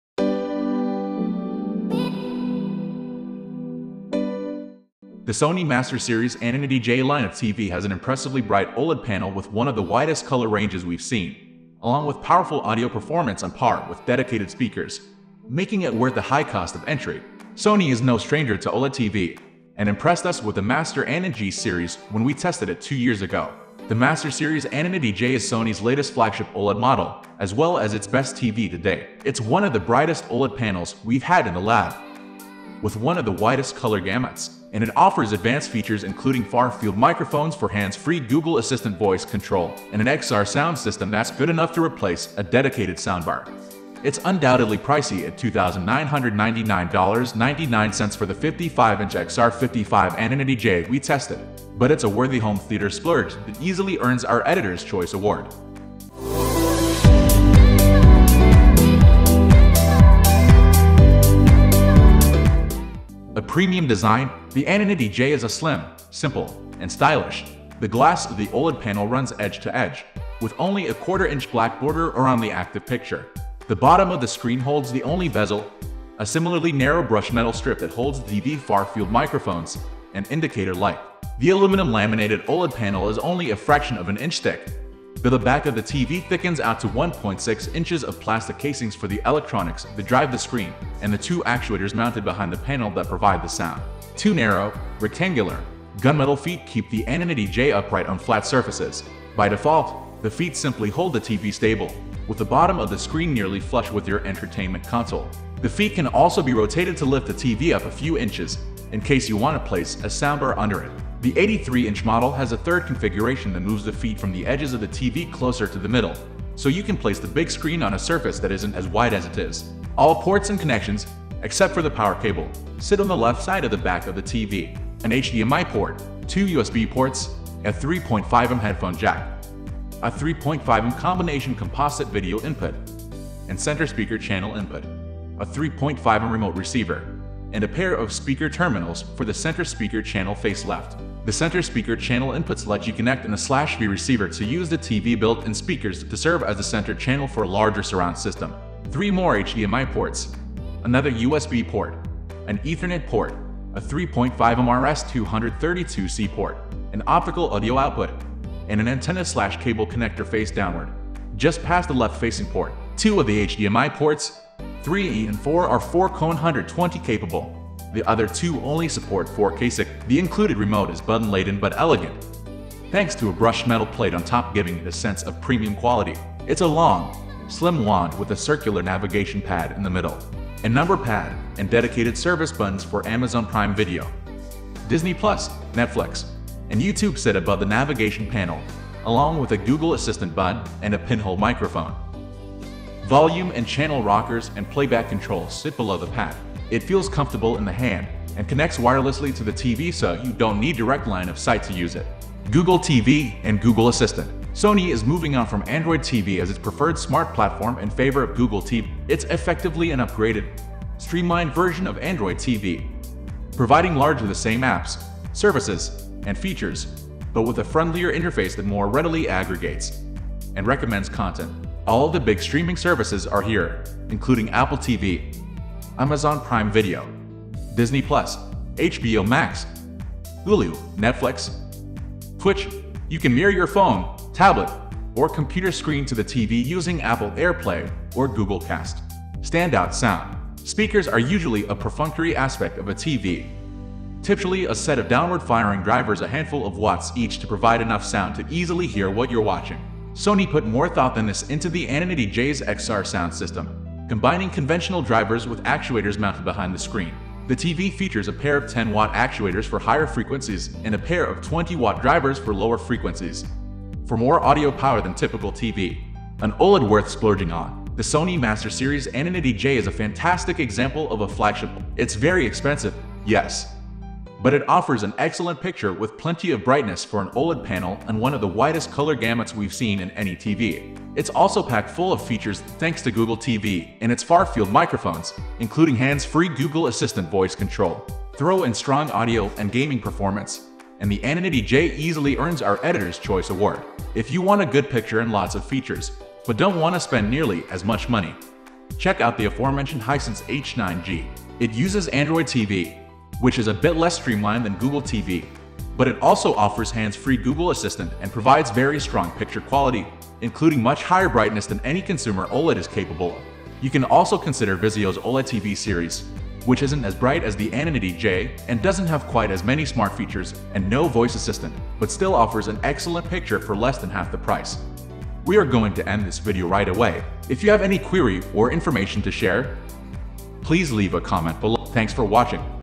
Sony Master Series XR 83A90J 4K HDR OLED TV Review: Should You Buy it? 2023
Sony is no stranger to OLED TVs, and impressed us with the Master A9G Series when we tested it two years ago. The Master Series A90J is Sony's latest flagship OLED model, as well as its best TV to date. It’s one of the brightest OLED panels, with one of the widest color gamut's.